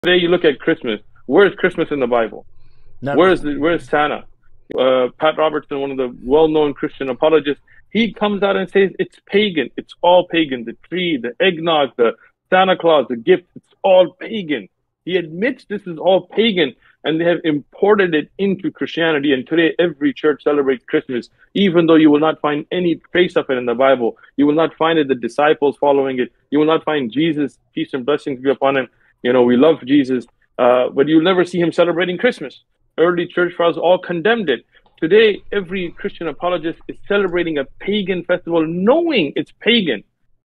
CHRISTMAS IS WHAT 😱
According to the bible Jesus was not
Christmas is not a part of cristianty, you can't find any thing about it in the bible, and according to the bible JESUS was not born in the 25th of December.

The Original Video at @OneMessageFoundation